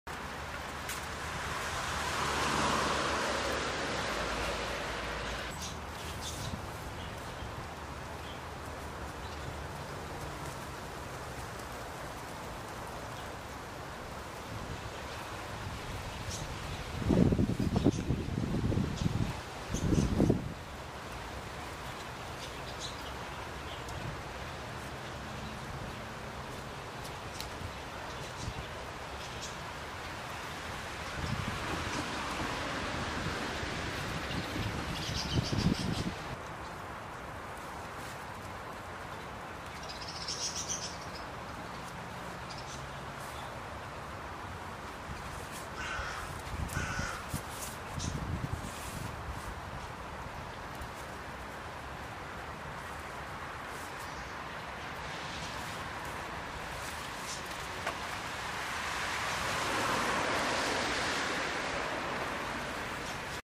The hunchback and notre Mia
Mia jumps up on my back and I stand up to see if I can walk around with her like that. Meanwhile Bob is not fazed by any of it.